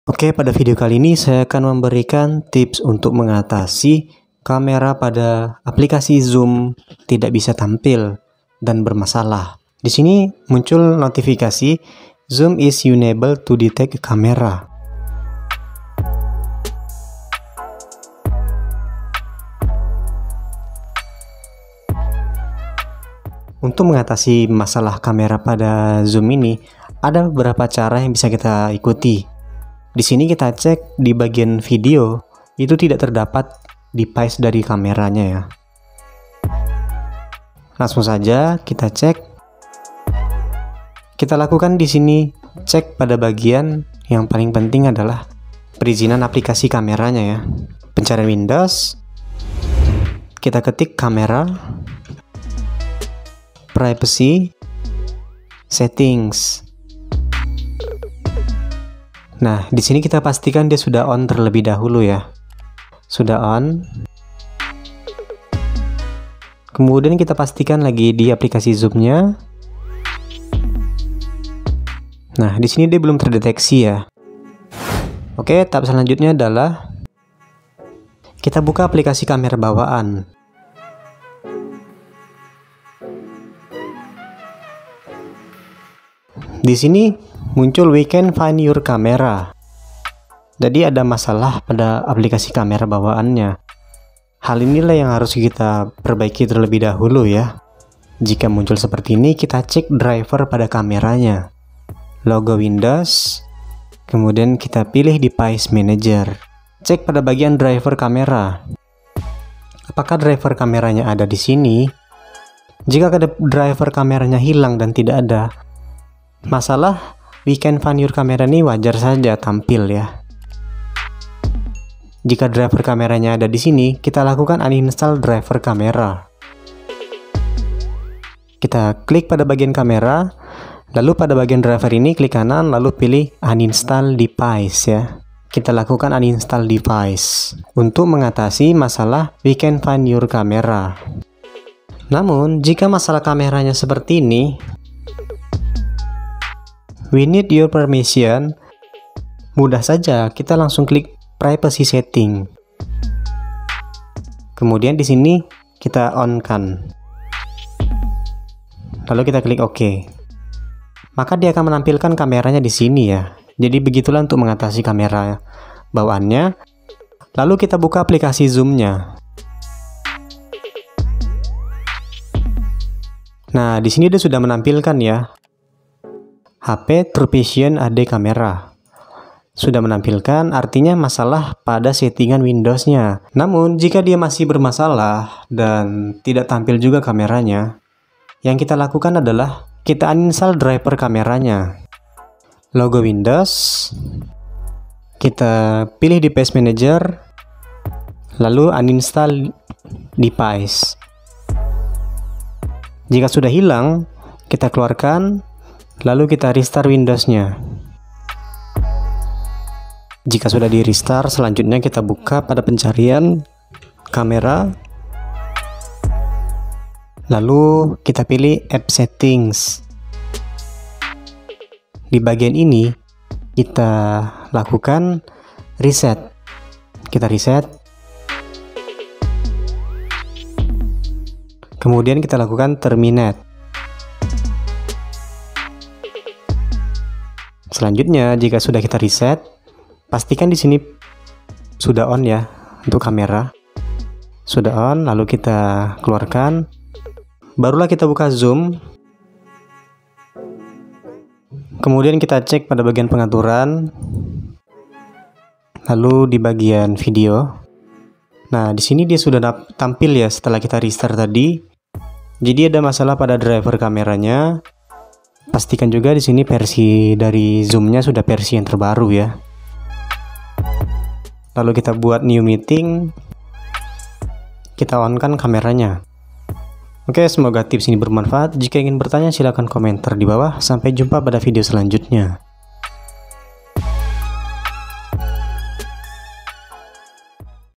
Tips Mengatasi Kamera di Zoom Meeting Tidak Tampil Pada Laptop Windows
Selamat datang kembali dichannel Habibi Edukasi Teknologi, kali ini saya akan memberikan Tips Mengatasi Kamera di Zoom Meeting Tidak Tampil Pada Laptop Windows. Dimana untuk masalah kamera tidak terdeteksi atau tidak tampil ketika kita menggunakan aplikasi zoom, pada dasarnya ini adalah masalah pengaturan dan kamera yang error dan hak akses perizinan dilaptop. Untuk selengkapnya bisa mengukuti di dalam video. Dan jika ingin bertanya silahkan komentar dibawah.
Record : iPhone Xr | Redmi Note 9
Editing Video : Wondershare Filmora X
Editing Thumbnail : Adobe Photoshop CC 2017